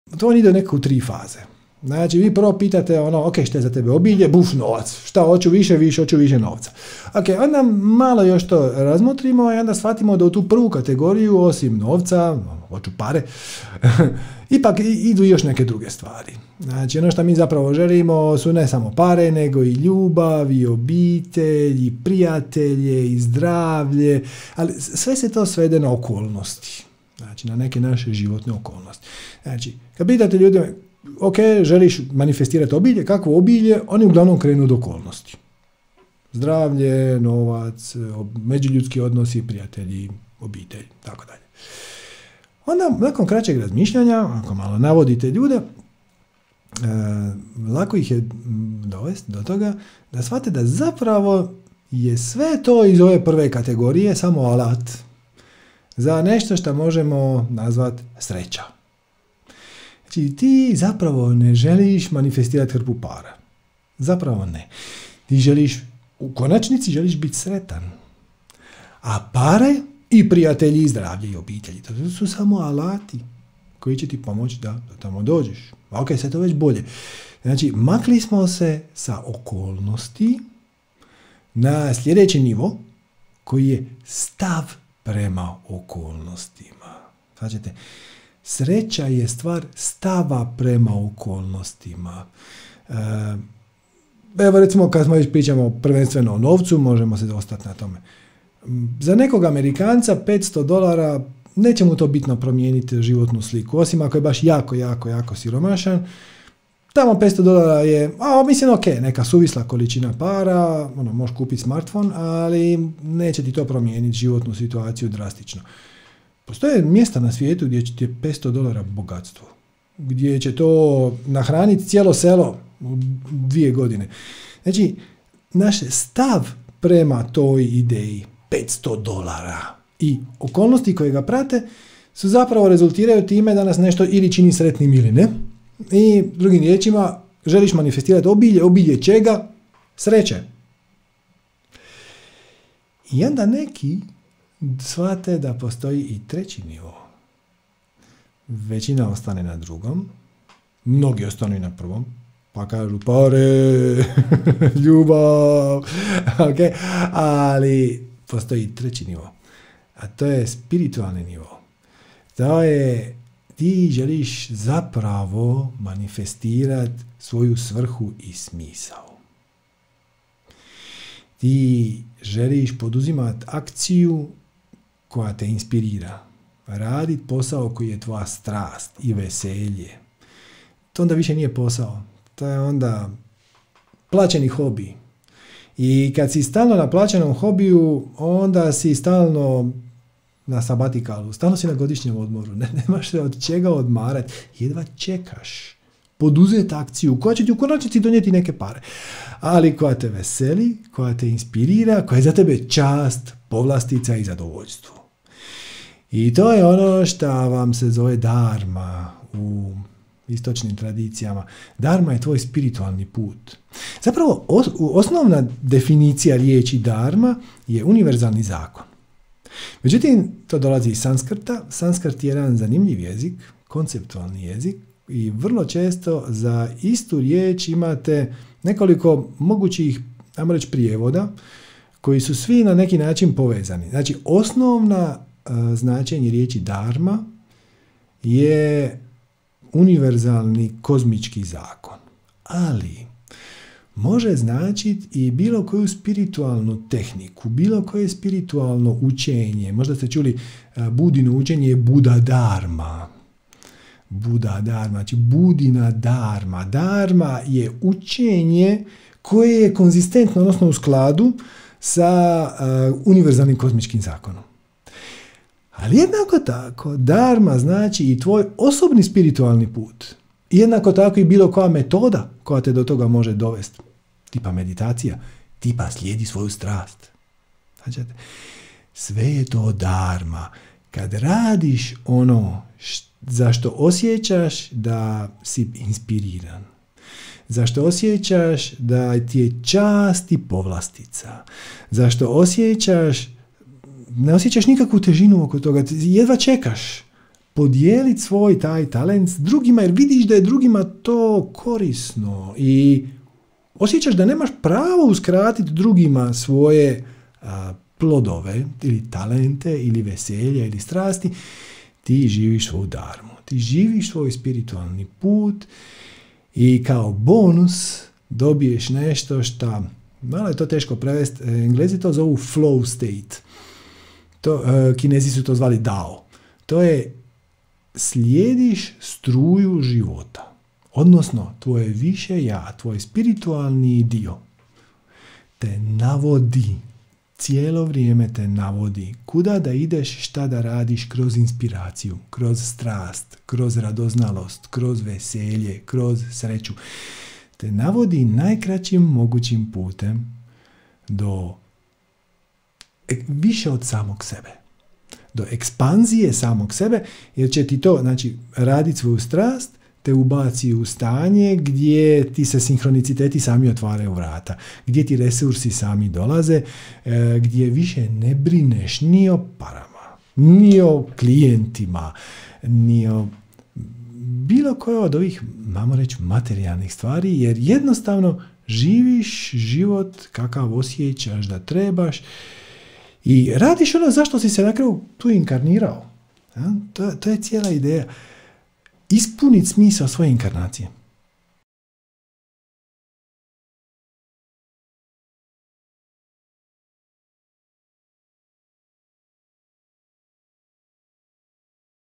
Srđan Roje: Zapravo ne želimo novac, pa čak ni sreću, nego nešto sasvim treće...
Dio masterclassa "Pare na Sunce" u kojem pričamo o manifestiranju novca. Prijave

Jedan od čestih komentara na ideju "manifestiranja novca" jer kako je novac nešto prljavo i spiritualno nekorisno. Ništa nije dalje od istine! A evo i zašto...